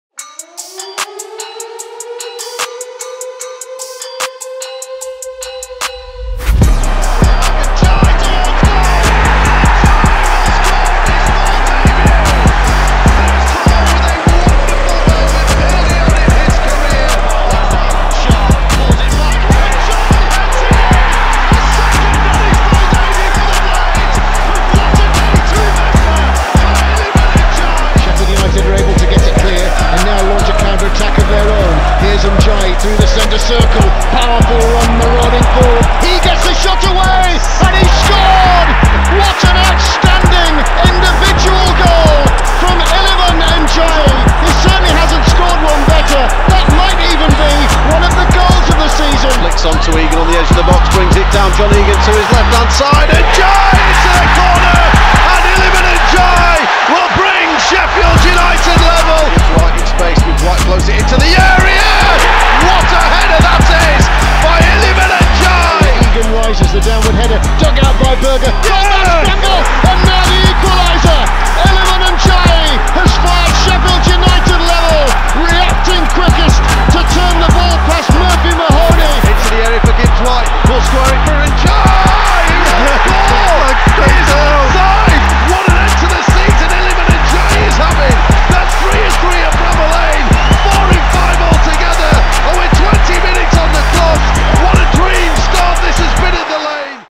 Iliman Ndiaye | All Goals, skills, highlights for Sheffield United 2021/22 | Senegal 🌪 🇸🇳
7 goals in a breakthrough season in the EFL Championship for from Sheffield and Senegal’s Iliman Ndiaye at the Blades. Watch all of his best bits on SUTV.